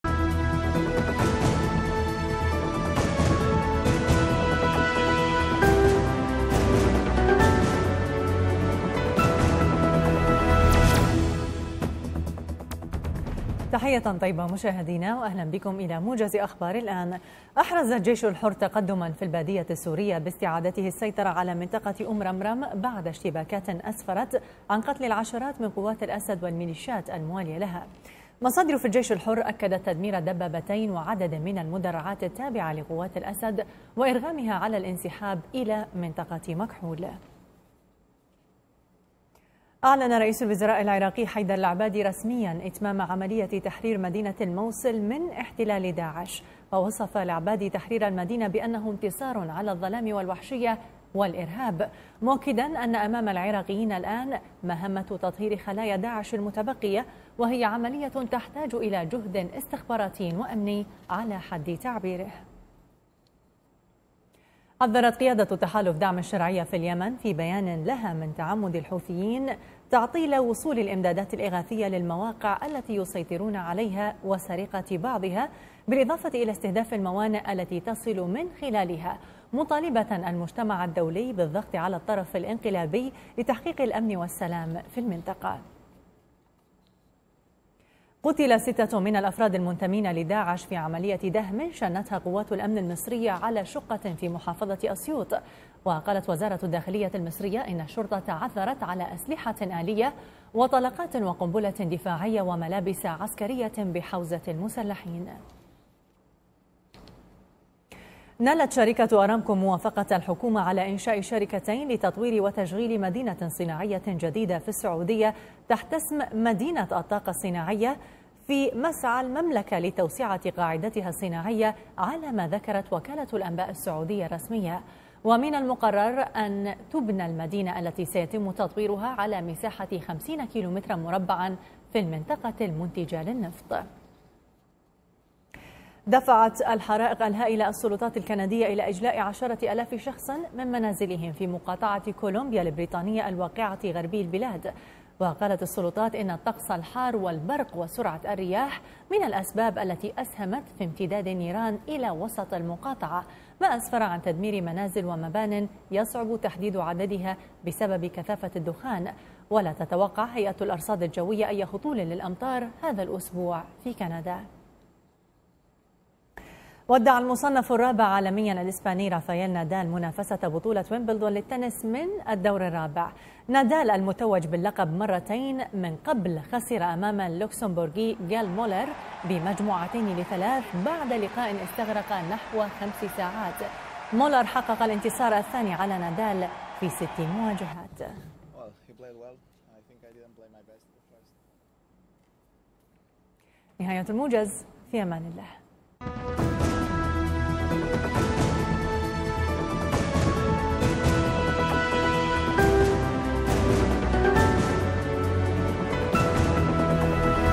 11-7-2017 | موجز الواحدة ظهراً.. لأهم الأخبار من
العناوين:
- المعارضة المسلحة تستعيد بعض مواقعها في البادية السورية
- التحالف يحذر من تعمد انقلابيي اليمن تعطيل وصول المساعدات
- الشرطة المصرية تقتل 6 إرهابيين في عملية دهم بمحافظة
- أرامكو تنال الموافقة على إنشاء شركتين لتطوير وتشغيل مدينة صناعية
- إجلاء 10 آلاف شخص بسبب حرائق في غرب
- نادل يخروج من الدول الرابع لبطولة ويمبلدون للتنس